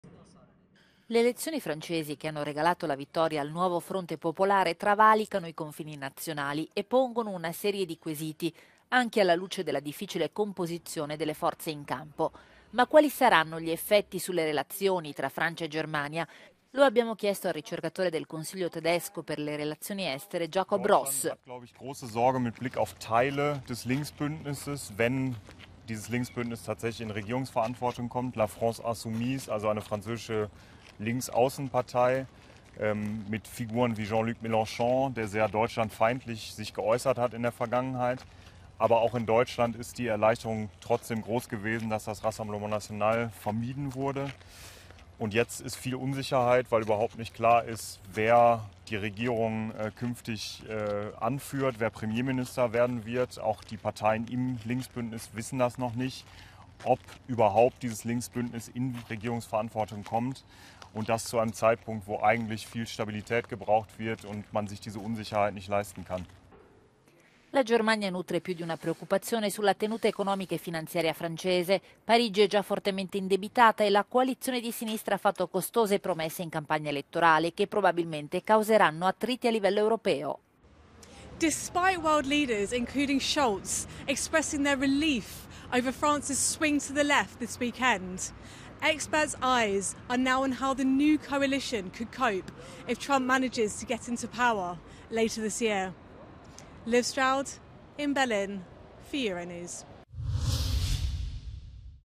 La Germania "preoccupata" per i risultati delle elezioni francesi
I risultati elettorali in Francia sono al centro di una riflessione che travalica i confini nazionali. Ma quali saranno gli effetti sulle relazioni della Francia con la Germania?